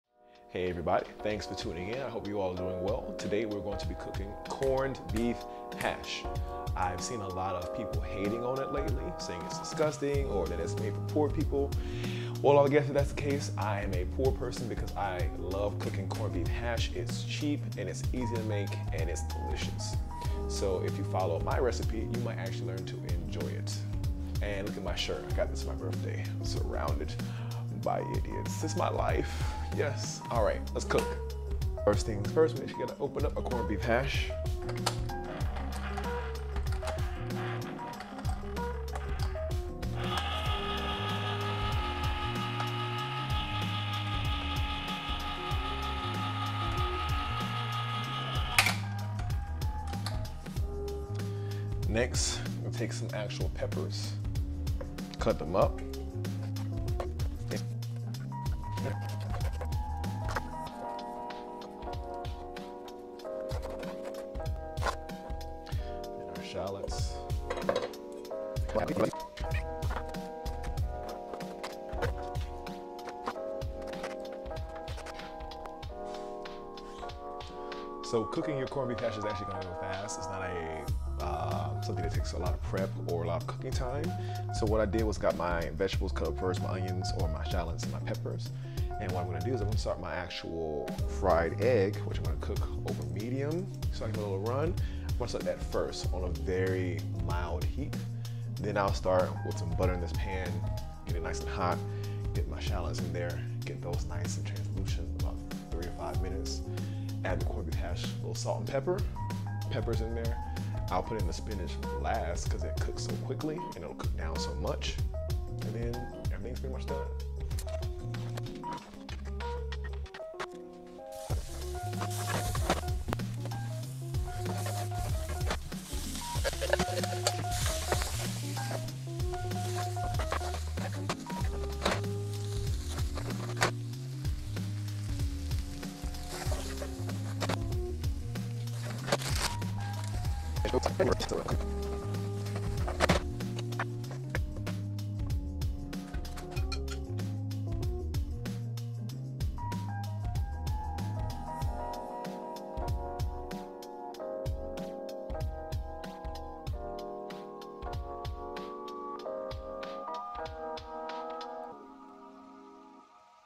Corned Beef Hash for breakfast!
This morning I decided to whip up some corned beef hash. Who else loves this easy treat for breakfast?!